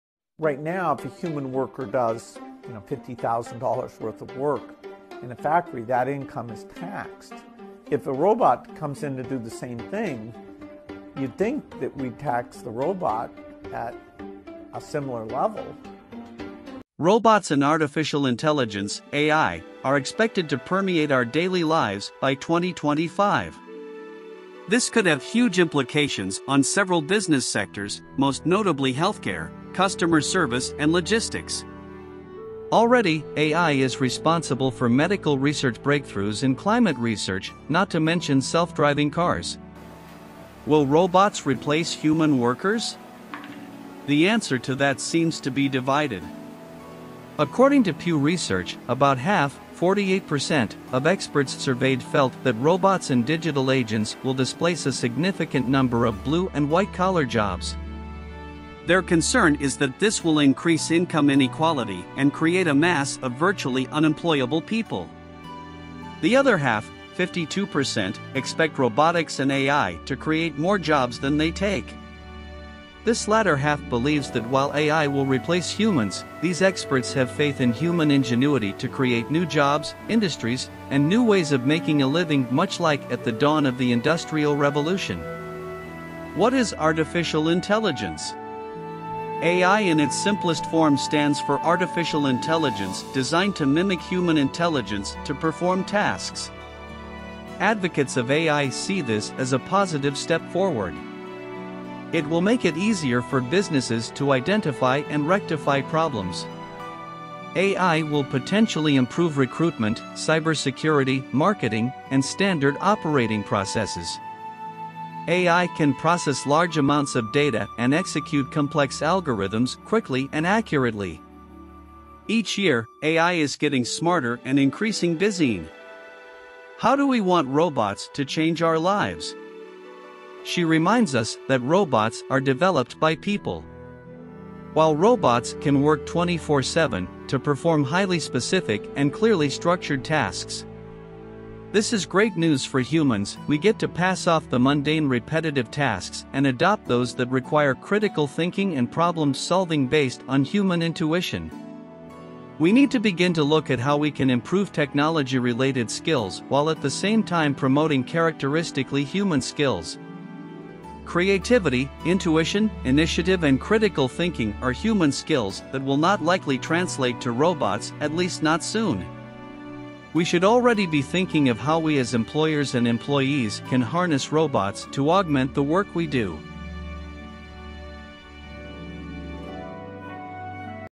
If AI And Robots Could Take The Place Of Humans ?
Robots and artificial intelligence (AI) are expected to permeate our daily lives by 2025. This could have huge implications on several business sectors, most notably healthcare, customer service and logistics. Already, AI is responsible for medical research breakthroughs and climate research, not to mention self-driving cars.

Will robots replace human workers?